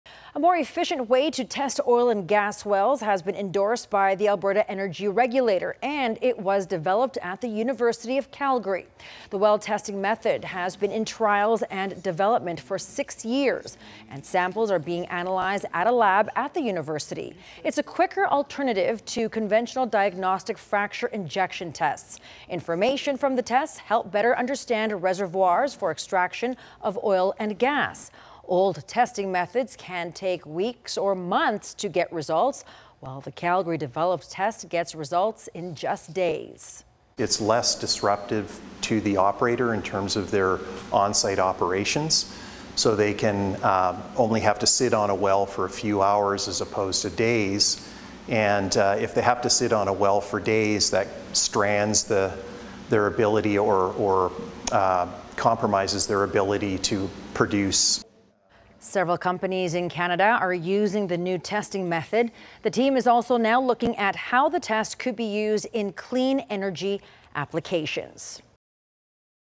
New oil and gas well testing technology
Scientists at the University of Calgary have developed a new way to test oil and gas wells and improve extraction.